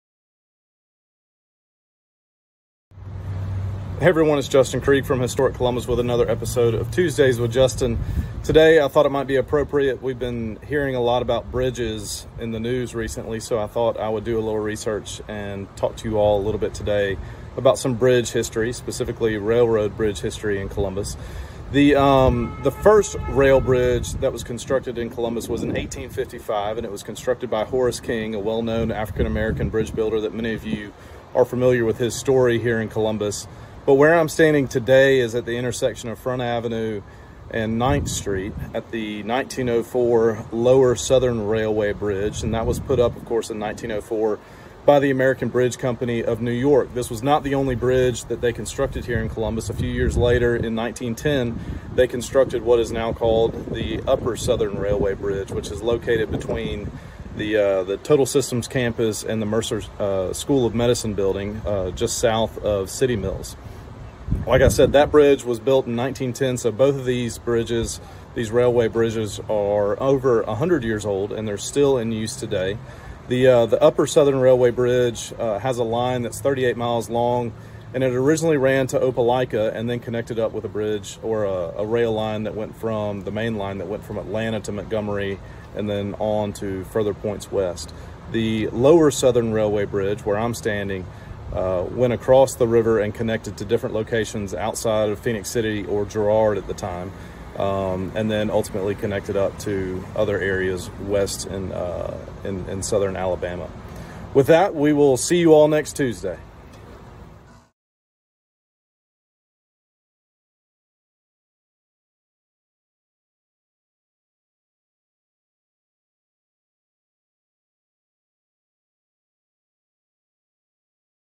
Tuesdays with Justin–Columbus Railroad Bridge History
Today's epidode of Tuesdays with Justin is all about Columbus railroad bridge history! 🚂

Tune in to learn more, and let us know what you think 📲